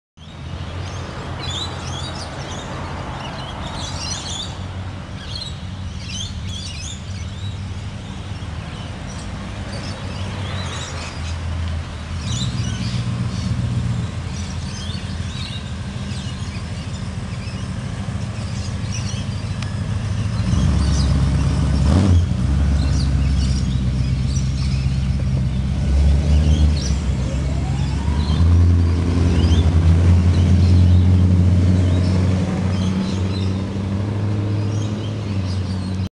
A bundaberg wedding car
Muscle on the streets of bundy. These were just a few of the cars at someones wedding reception.